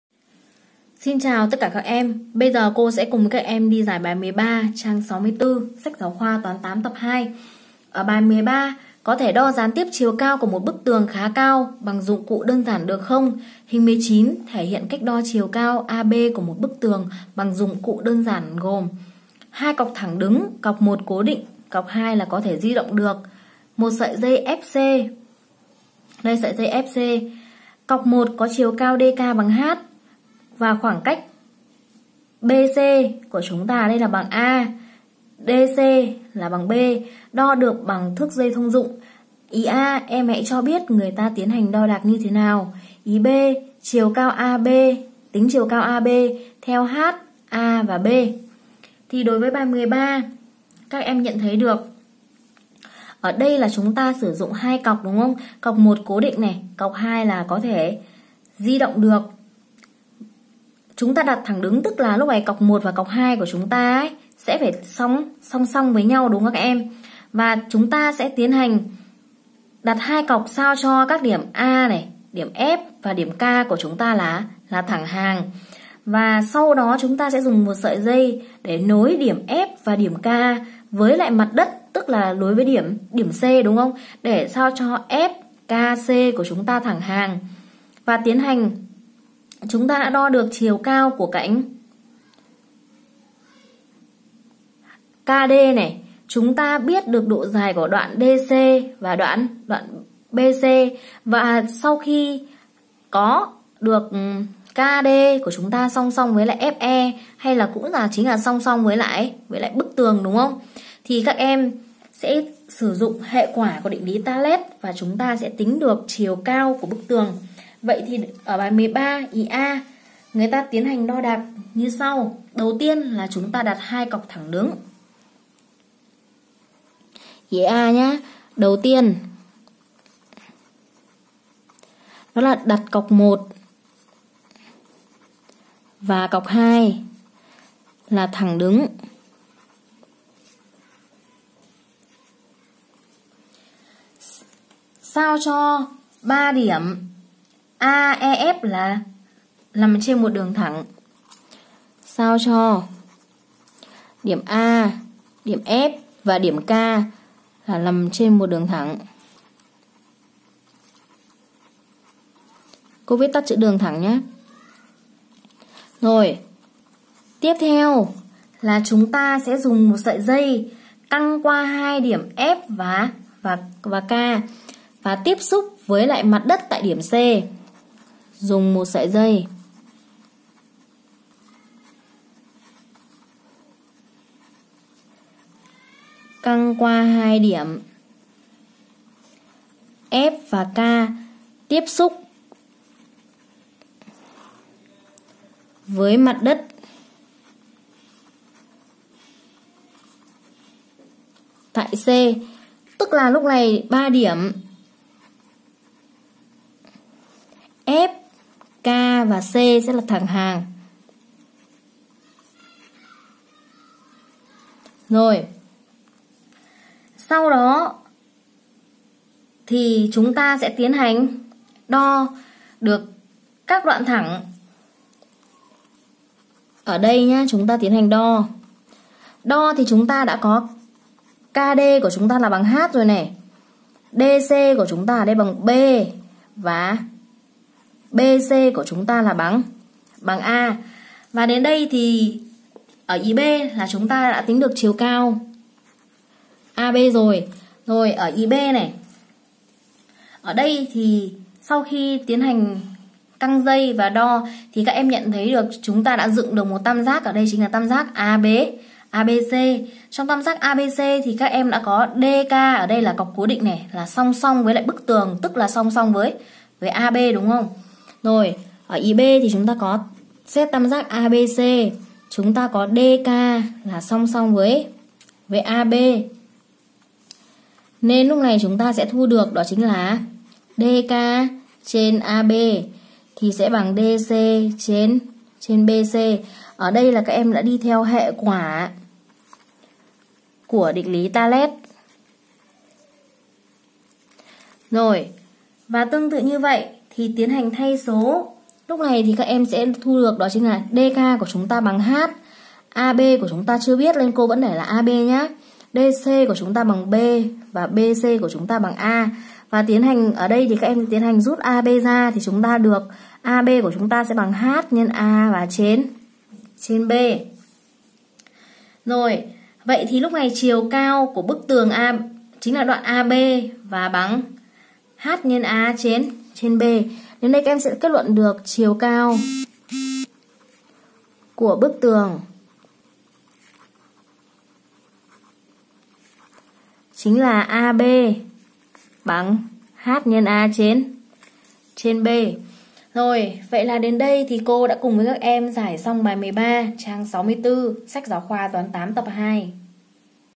Giải bài 13 trang 64 SGK Toán 8 tập 2
Có thể đo gián tiếp chiều cao của một bức tường bằng dụng cụ đo đơn giản được không?
  Hình 19: thể hiện cách đo chiều cao  AB  của một bức tường bằng các dụng cụ đơn giản gồm:
  Hai cọc thẳng đứng và sợi dây  FC , Cọc 1 có chiều cao  DK= h . Các khoảng cách  BC= a, DC= b  đo được bằng thước thông dụng.
  a) Em hãy cho biết người ta tiến hành đo đạc như thế nào ?
  b) Tính chiều cao  AB  theo  h, a, b . .